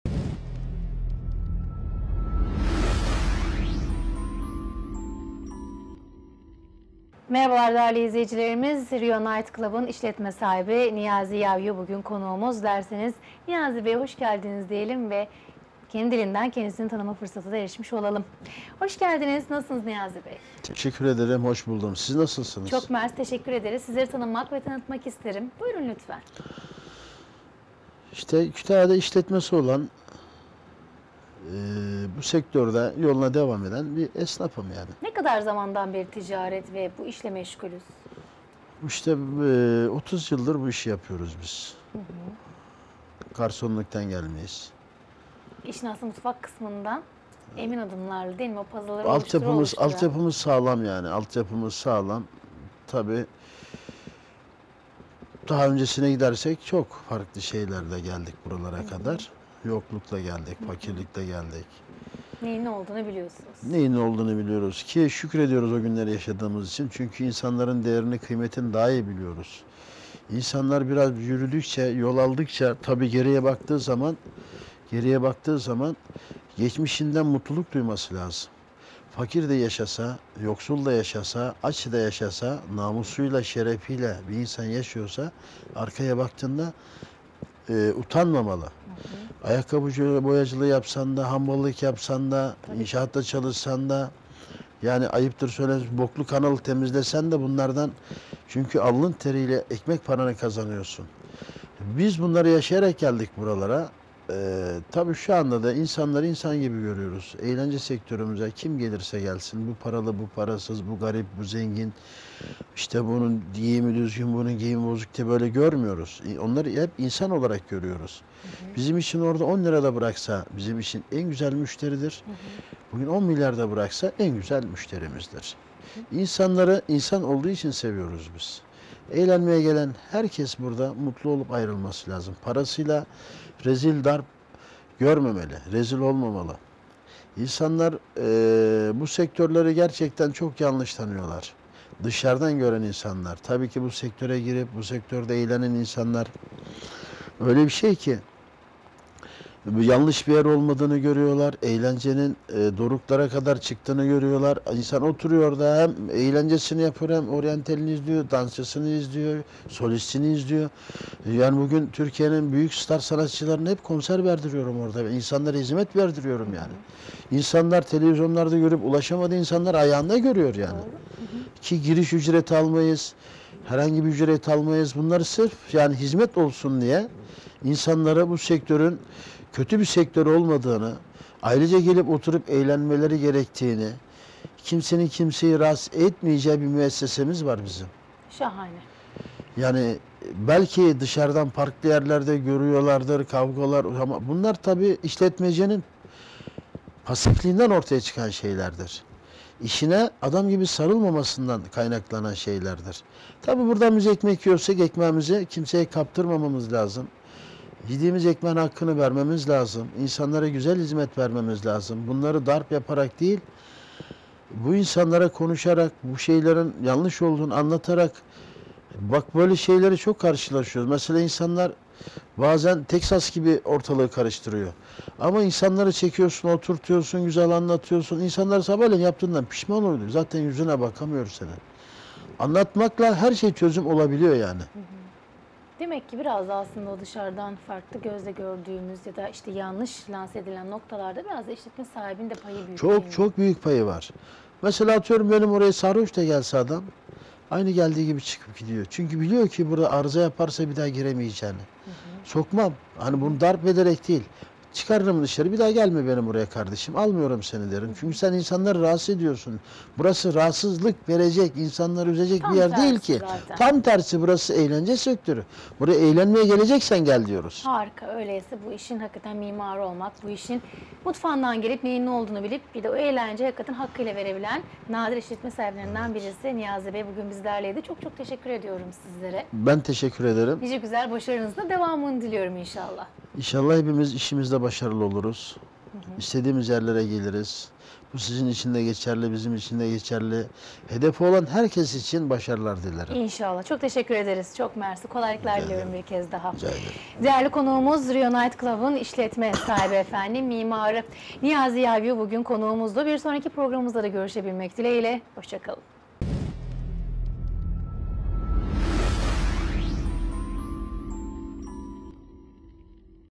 NİYAZİ YAYVU POSTA KOBİ
Sektörün marka isimlerinden ''Rio Naytklap'' işletme sahibi Niyazi YAYVU  vermiş olduğu hizmetleri Posta Kobi'ye anlattı.